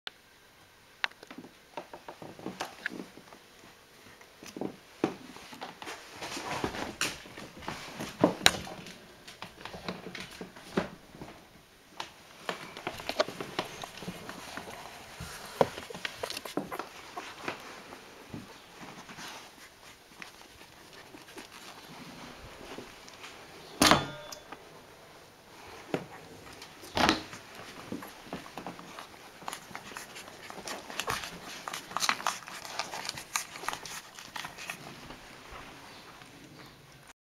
Le retour du travail
Vidéo servant à l'illustration de l'histoire des chiots de Violine que je publie dans mon blog